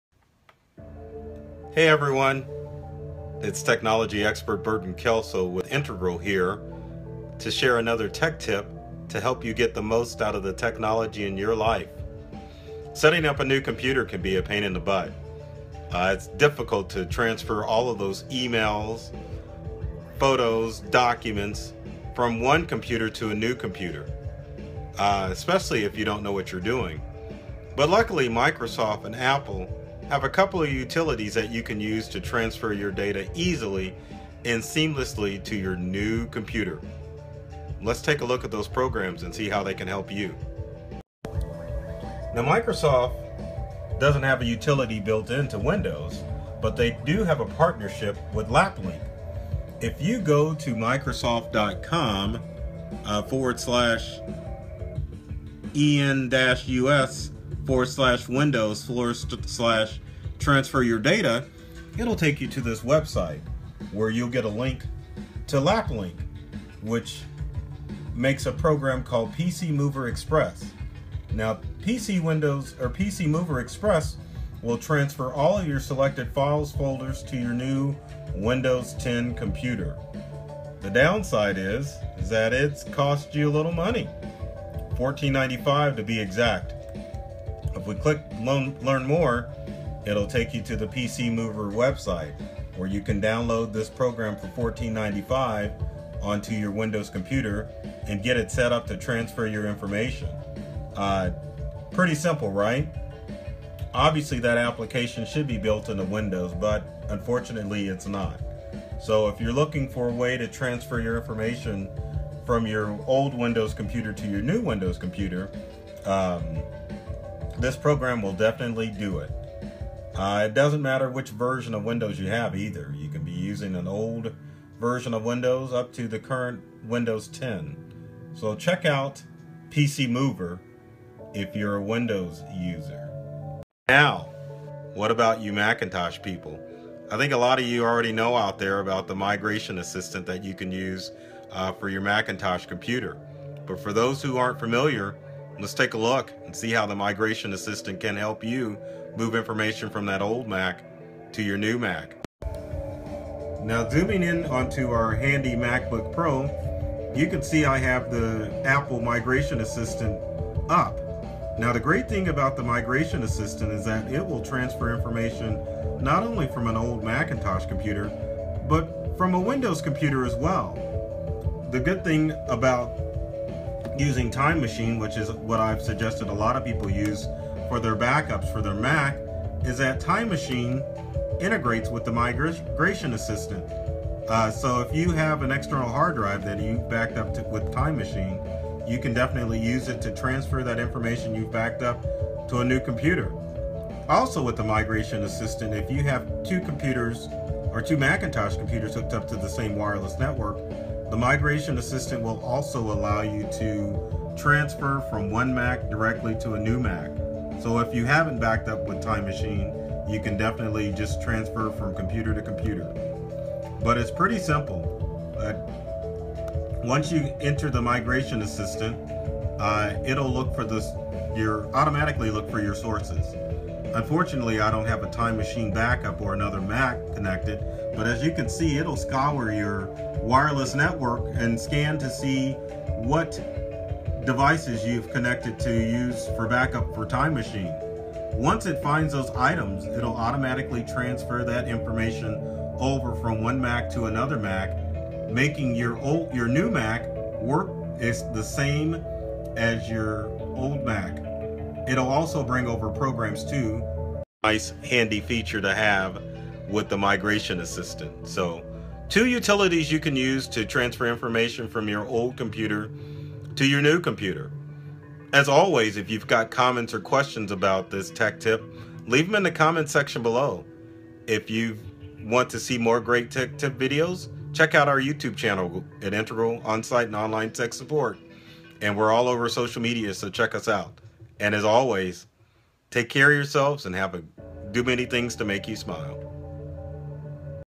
Tech Tips for Non Tech People | How to Quickly Transfer Your Data and Settings to a New Computer
In this video tip, Technology Expert Burton Kelso shows what programs to use to transfer your information to a new computer.